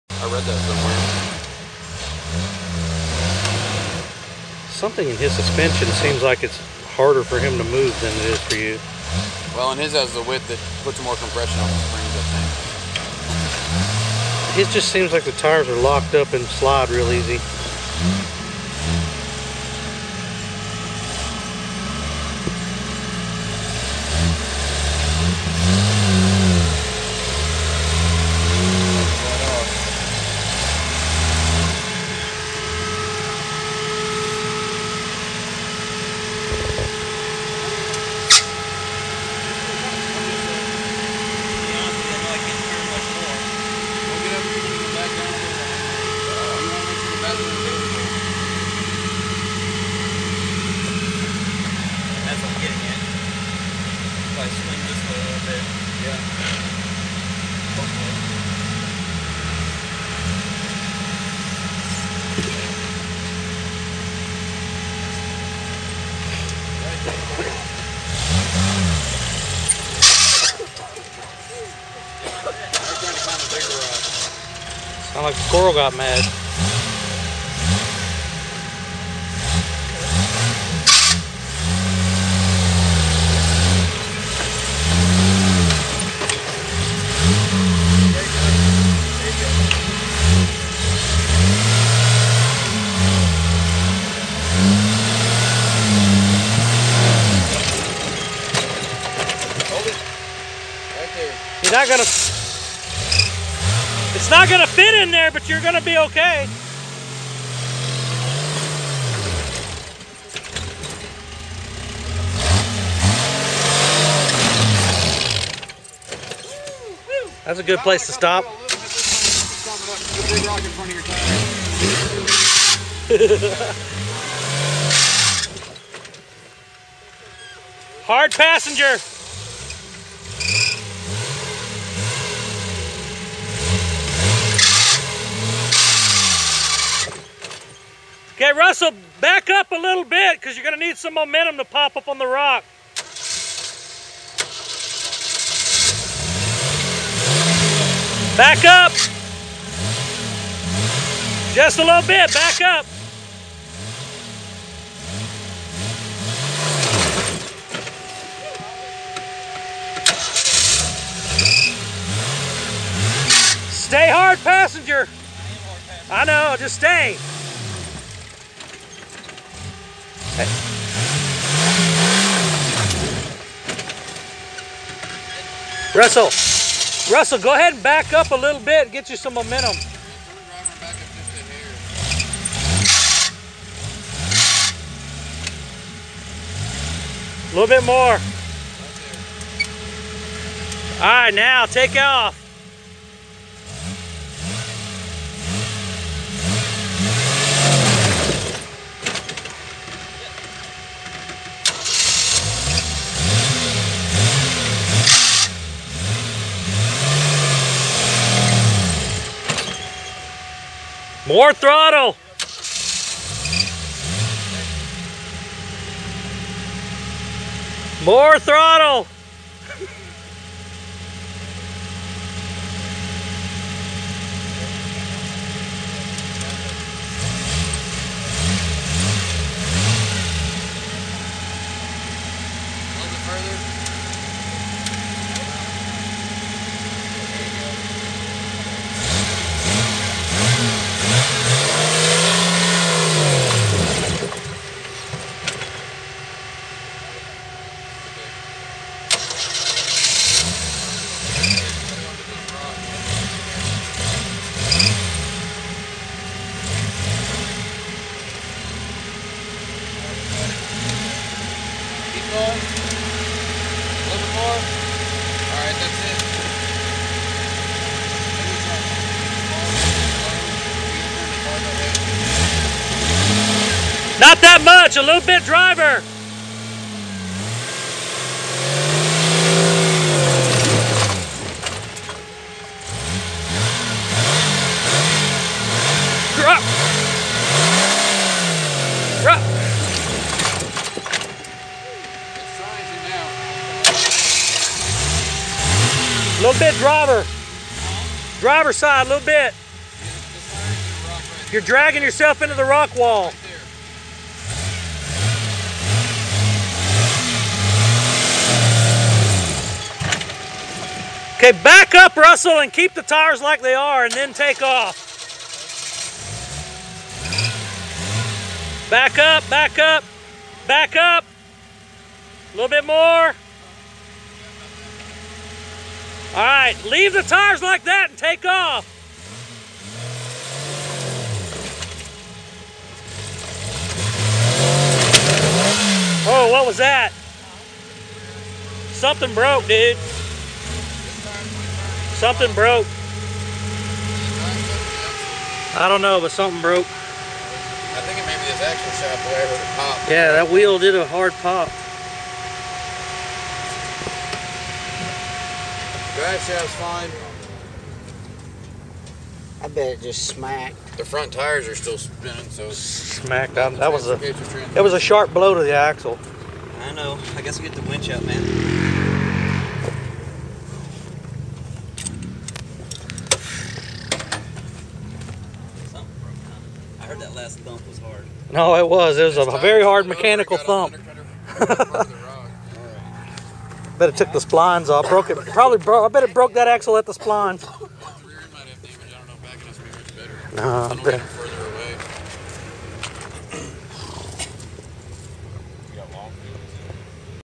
Big Boy Ledge eats Earl's lunch 10-16-2010 at Shiloh Ridge in Alto, Texas
Russell piloting his jeep Earl up Big Boy Ledge at Shiloh Ridge offroad park in Alto, Texas on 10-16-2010 for the Halloween Ride weekend.

This is one of the most difficult obstacles in the entire park. Earl suffers a broken transfer case output shaft and then a broken front hub in this attempt which ended the weekend for Earl. There was much carnage on this particular rock climb as a few other crawlers bit the dust attempting it also, even some of the high dollar big boys broke things here.

Maybe Russell can make this climb another day but this particular day the climb won.